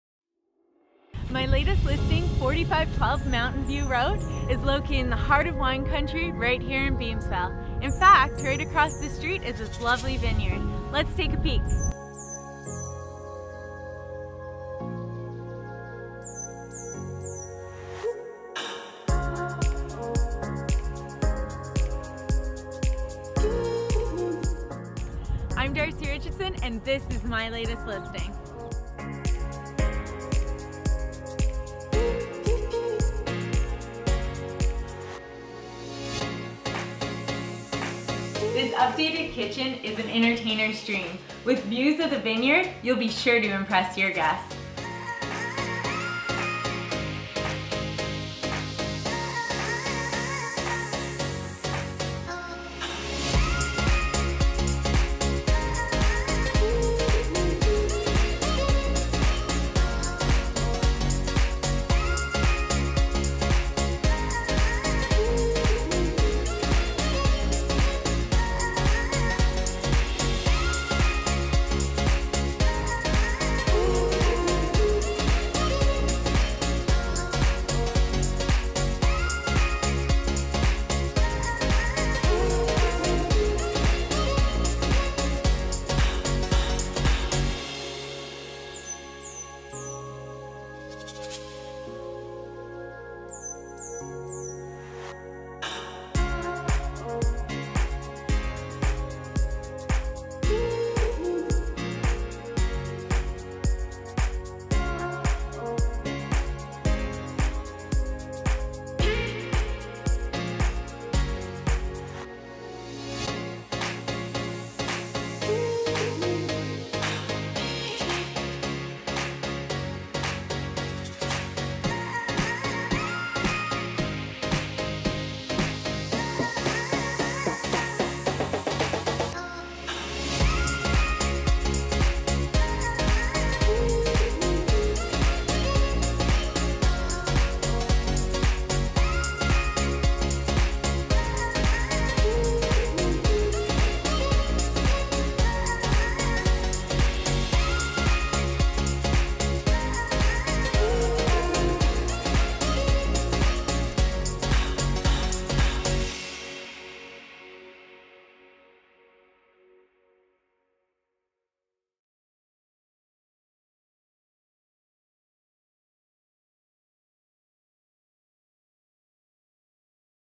***SOLD*** 4512 Mountainview Road, Beamsville $599,900
First time offered in almost 30 years, this one is a keeper! 4512 Mountainview Road features a sprawling bungalow perfectly situated on 1.11 acres with an in-ground saltwater pool in lovely Beamsville. Tranquil is the best way to describe this property, from the warm feeling when you enter the home, to the sun porch where you will be able to envision many long nights playing games surrounded by family, and the massive yard backing onto a creek where you can garden and take in nature's beauty on your own time. It really doesn't get much better than this, your own oasis nestled in one of Southern Ontario's most sought-after areas, located just 6 minutes from the QEW, surrounded by wineries, all with city water and natural gas. The lovingly maintained home has been spoiled with the following recent updates: fabulous kitchen renovation (2011 - take note of the solid maple painted cabinetry!), rear patio (2016), roof (2008), family room renovation (2009), lower bathroom renovation (2009), living room built-in (2013), pool liner and pump (2010). Don't wait, this is your chance to own a slice of wine country!